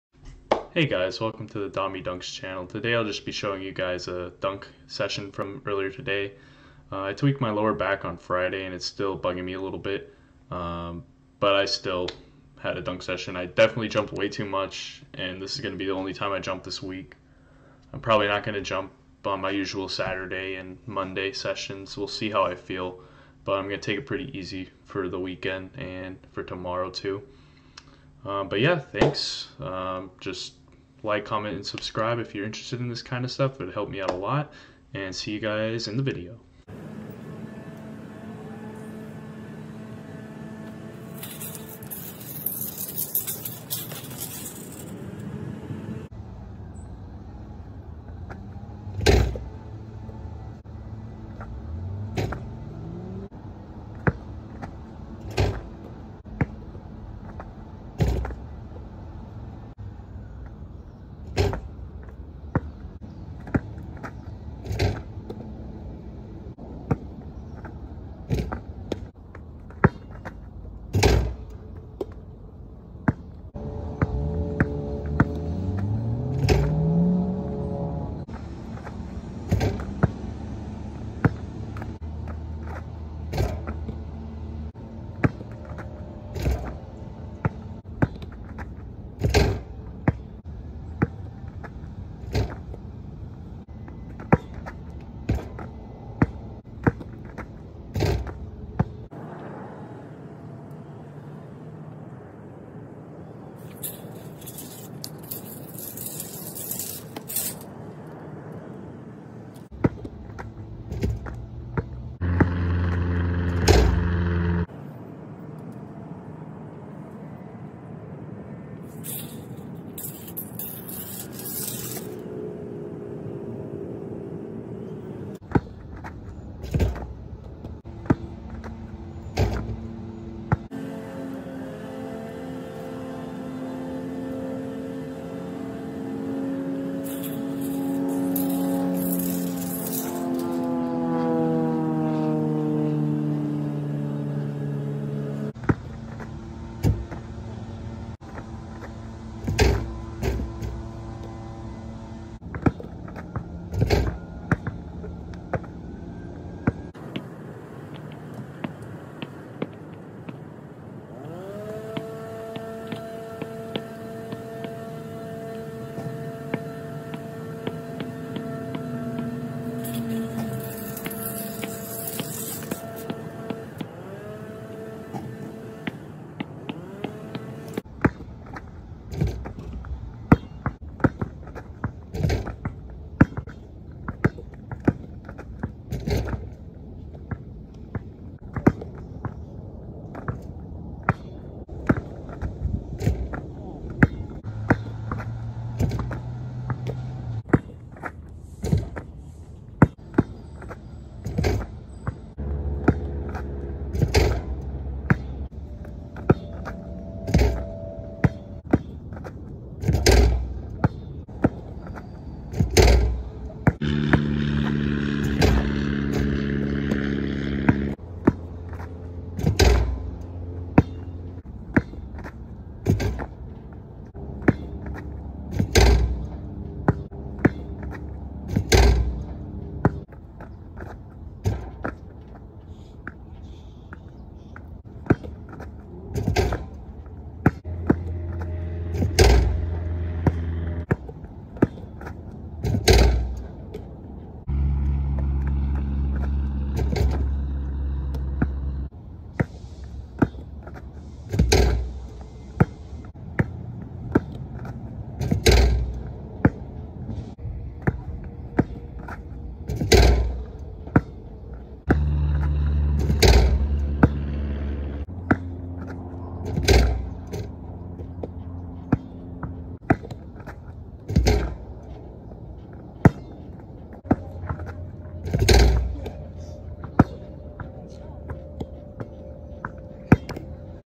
Dunk Journey Ep. 30: How high can I dribble dunk?!
Tweaked my lower back on Friday :(. Thankfully this week was my deload week anyway, so very light lifting! I jumped way too much in this session considering my back tweak, hopefully, I didn't make things worse.
  Probably no jumping on Saturday I'll see how my back feels.
Height: 6'
Standing Reach: 7' 9"
Weight: 177 lbs (80kg)  Thanksgiving weight (plus bulking)!
Standing Vertical ~ 25"
Max Vertical ~ 30-31"